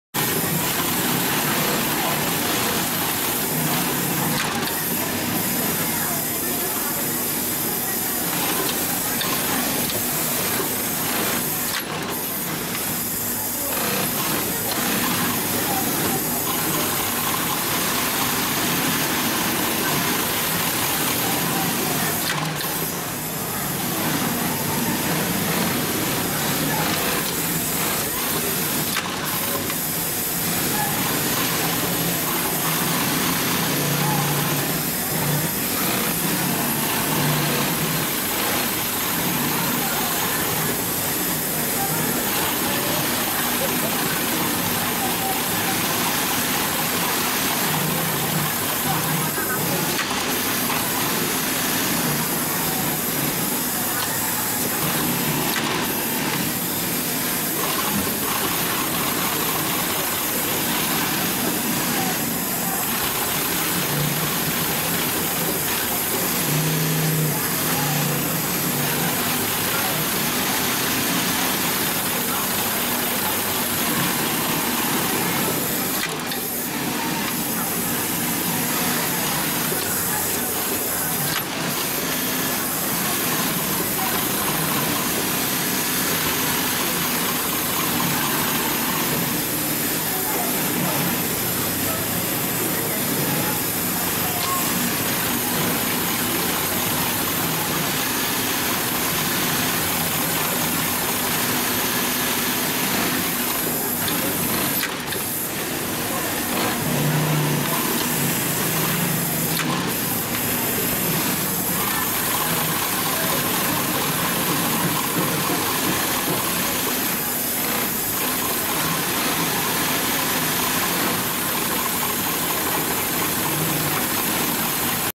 Sofa sewing machine for thick threads decorative stitches
HIGHTEX7204-370DP Heavy duty post bed triple feed double needle lockstitch sewing machine with 2 large Barrel shuttle hook for ultra strong thread decorative seams on leather.